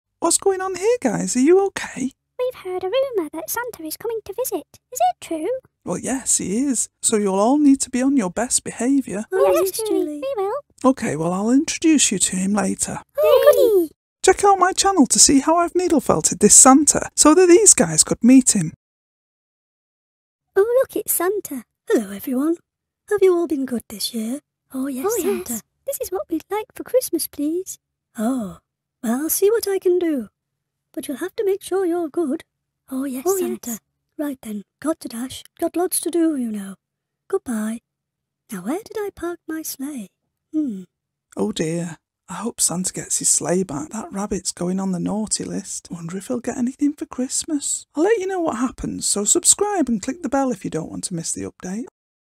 Santa visits the Felted Friends Animation
These are some of my attempts at animating my needle felted creations.  For full instructions on how I needle felted Santa watch this video: